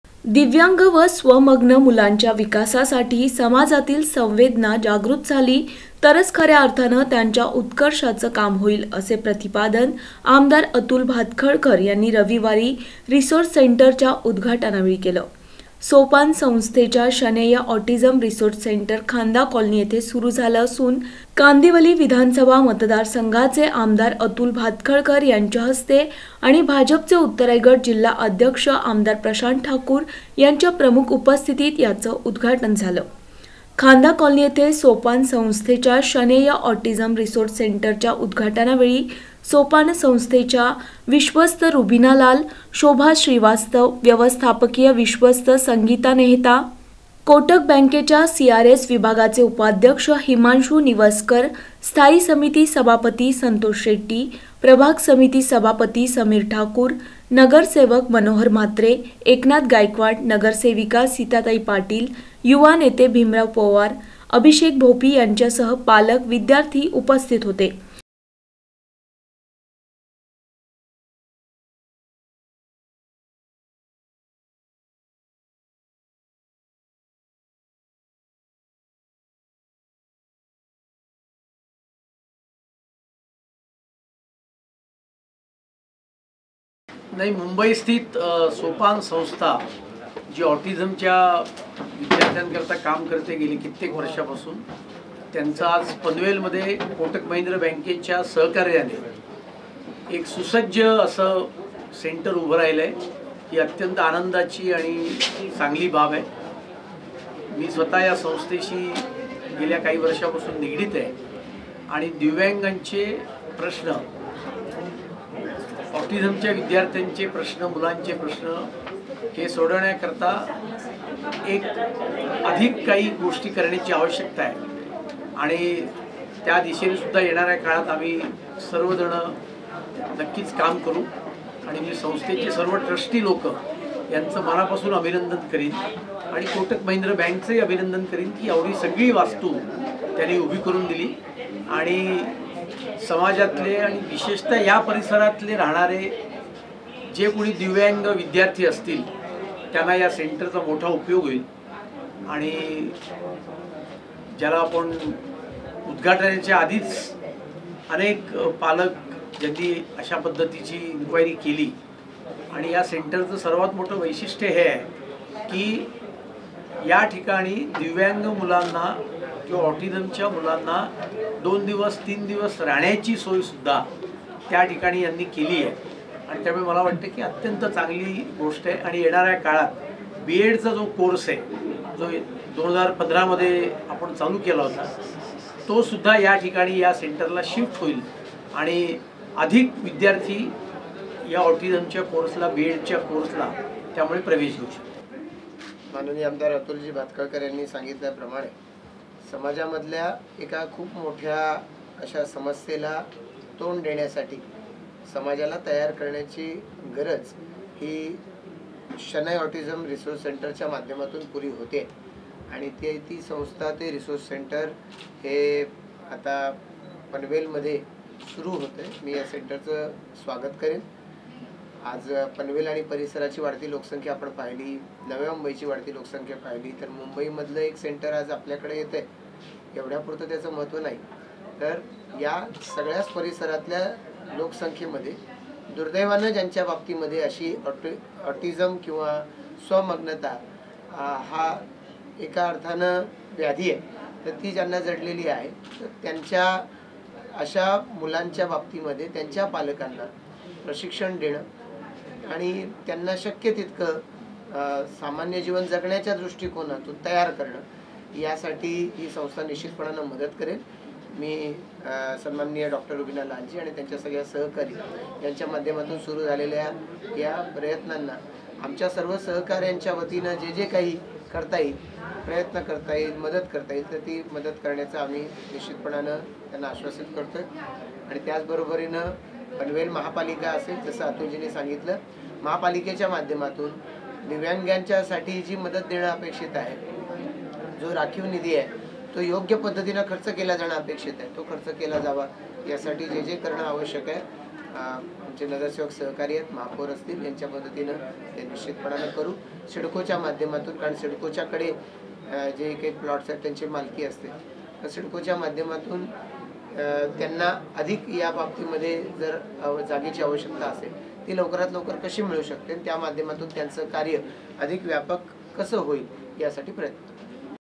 सोपान संस्थेच्या शनय ऑटिझम रिसोर्स सेंटरचे आमदार अतुल भातखळकर यांच्या हस्ते उद्घाटन !
दिव्यांग व स्वमग्न मुलांच्या विकासासाठी समाजातील संवेदना जागृत झाली तरच खऱ्या अर्थाने त्यांच्या उत्कर्षाचे काम होईल, असे प्रतिपादन आमदार अतुल भातखळकर यांनी आज (दि. ०५) खांदा कॉलनी येथे केले. सोपान संस्थेच्या शनय ऑटिझम रिसोर्स सेंटरचे उद्घाटन कांदिवली विधानसभा मतदार संघाचे आमदार अतुल भातखळकर यांच्या हस्ते आणि भाजपचे उत्तर रायगड जिल्हाध्यक्ष व पनवेल विधानसभा मतदार संघाचे आमदार प्रशांत ठाकूर यांच्या प्रमुख उपस्थितीत पार पडले. त्यावेळी ते बोलत होते.
           या कार्यक्रमास सोपान संस्थेच्या विश्वस्त रुबिना लाल, शोभा श्रीवास्तव, व्यवस्थापकीय विश्वस्त संगिता मेहता, कोटक बँकेच्या सीआरएस विभागाचे उपाध्यक्ष हिमांशू निवसरकर,  स्थायी समिती सभापती संतोष शेट्टी, प्रभाग समिती सभापती समीर ठाकूर, नगरसेवक मनोहर म्हात्रे, नगरसेवक एकनाथ गायकवाड, नगरसेविका सिता पाटील, युवा नेते भीमराव पोवार, अभिषेक भोपी, यांच्यासह पालक, विद्यार्थी उपस्थित होते !